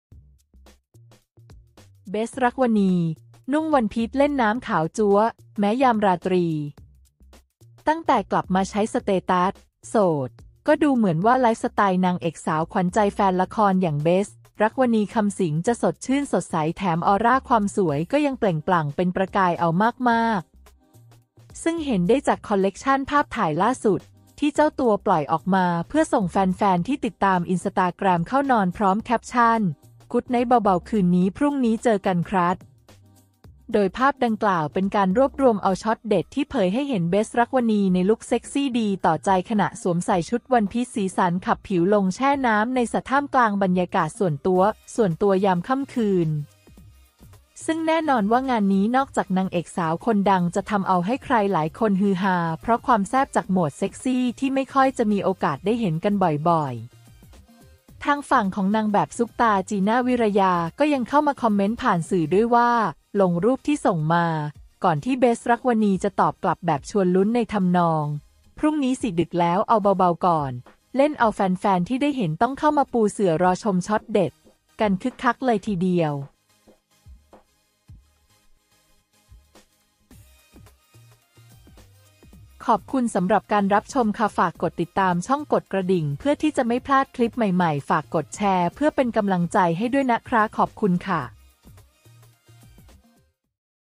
"เบสท์ รักษ์วนีย์" นุ่งวันพีซเล่นน้ำขาวจั๊วะ! แม้ยามราตรี
"เบสท์ รักษ์วนีย์" นุ่งวันพีซเล่นน้ำ ขาวจั๊วะ! แม้ยามราตรี

ขอบคุณสำหรับการรับชมค่ะ ฝากกดติดตามช่อง กดกระดิ่ง เพื่อที่จะไม่พลาดคลิปใหม่ๆ ฝากกดแชร์ เพื่อเป็นกำลังใจให้ด้วยนะคะ ขอบคุณค่ะ